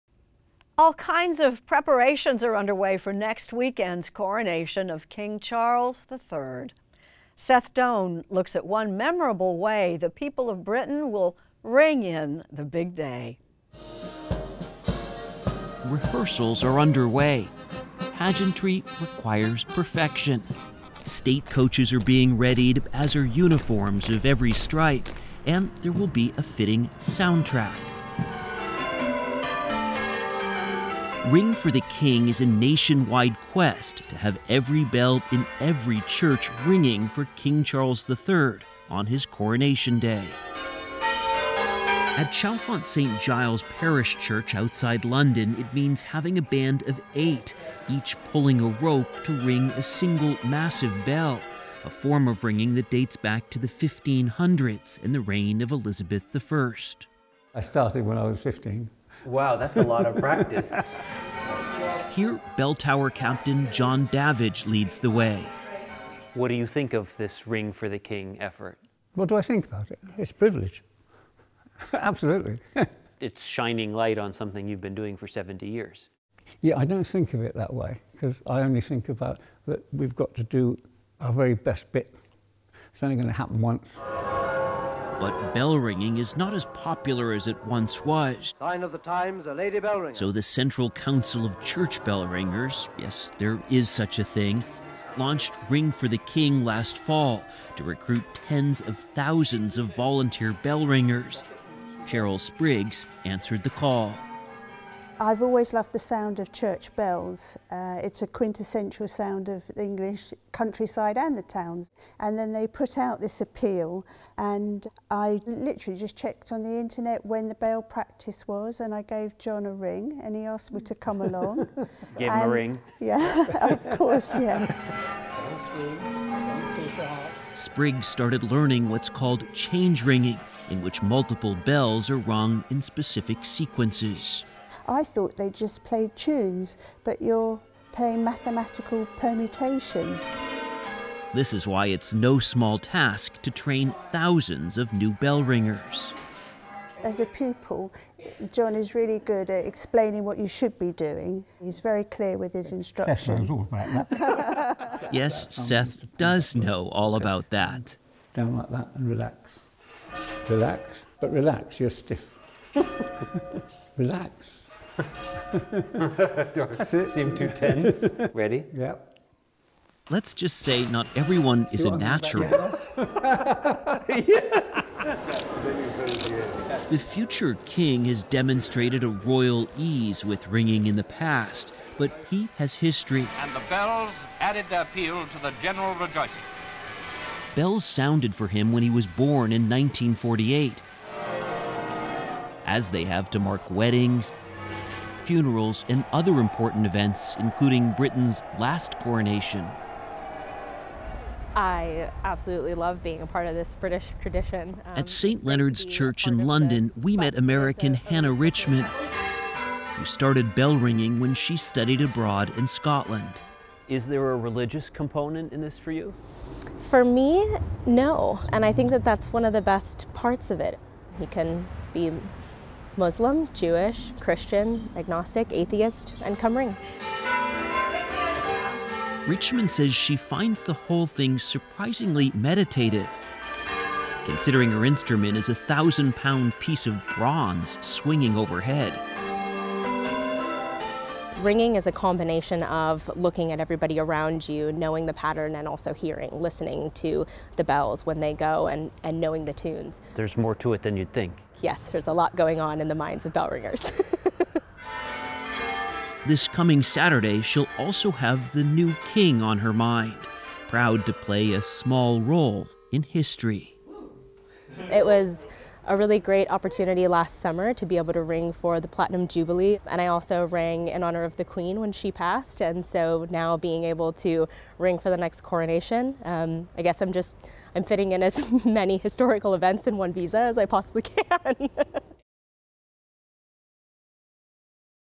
"Ring for the King": Bell ringers prepare for coronation of King Charles III
On Coronation Day for Britain's new king, bells in churches across the United Kingdom will be rung. Bell ringing is a tradition that dates back centuries. But a shortage of bell ringers meant an appeal was made for volunteers. Correspondent Seth Doane gets an earful as he checks out rehearsals for the big day.